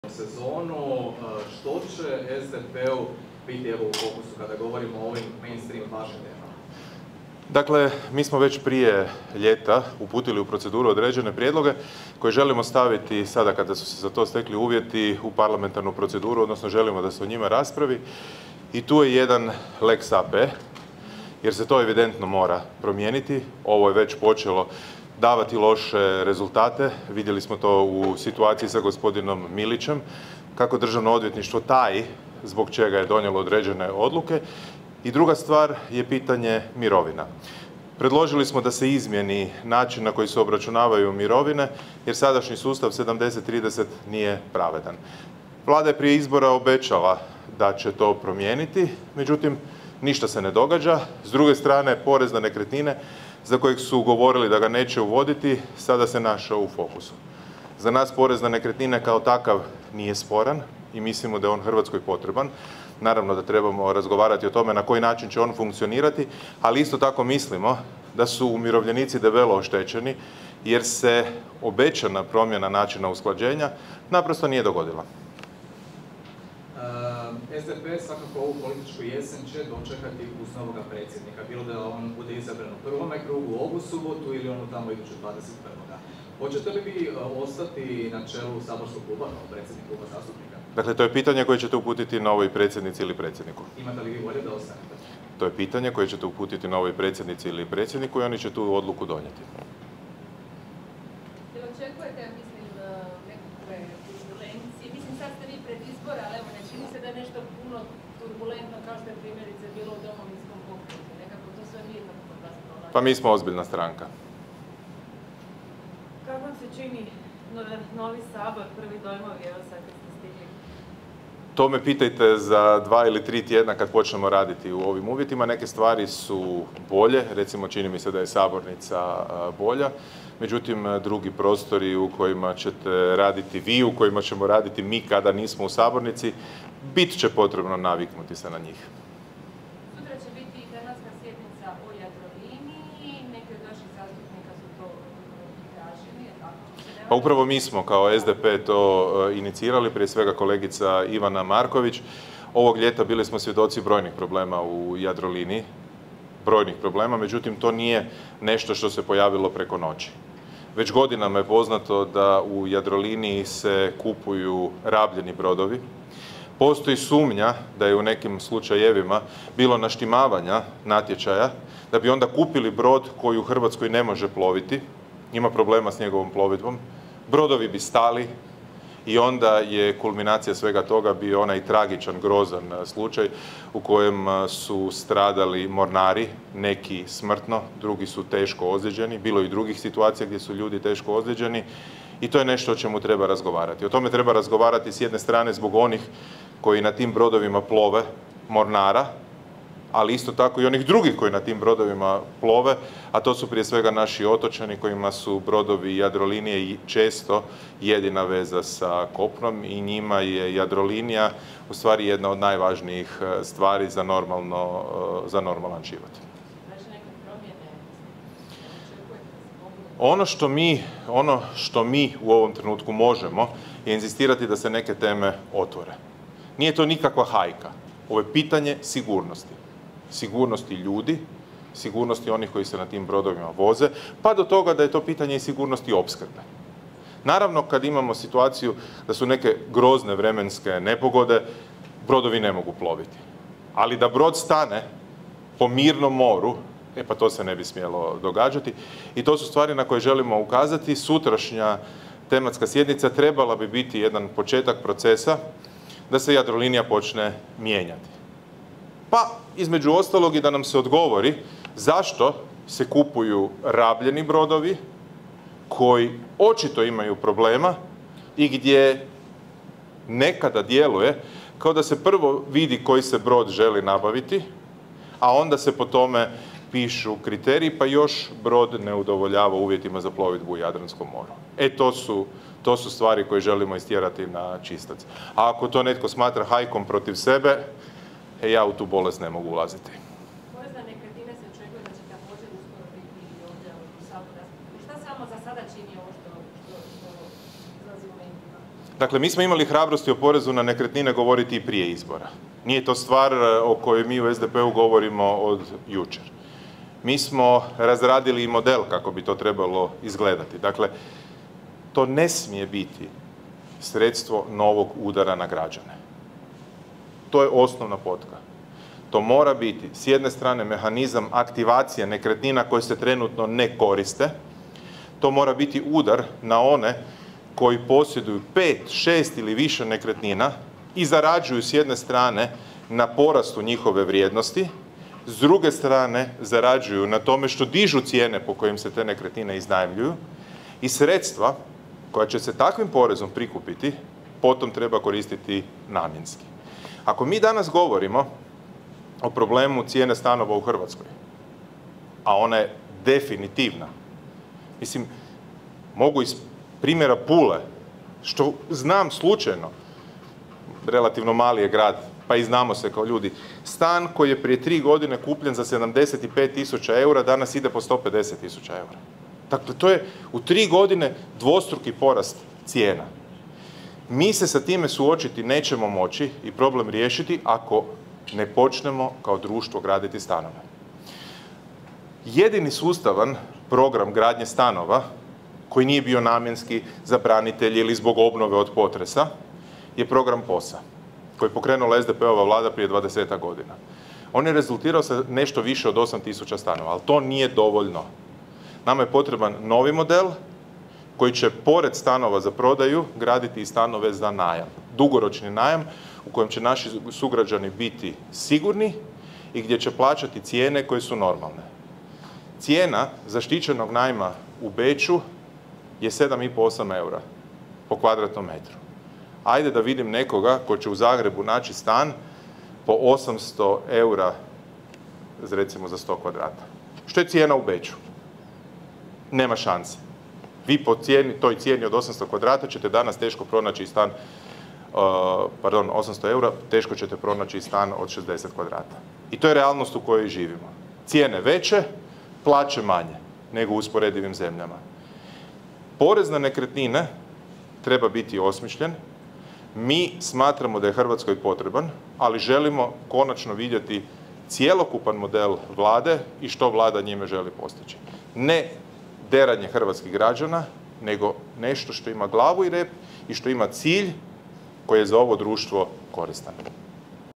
Grbin: "O Jadroliniji treba razgovarati zbog mornara, ali i zbog otočana"
Prvi put u novom prostoru vojarne na Črnomercu odrzana je sjednica Predsjedništva Hrvatskoga sabora s predsjednicima klubova zastupnika.  Nakon sjednice konferenciju za medije održao je Peđa Grbin.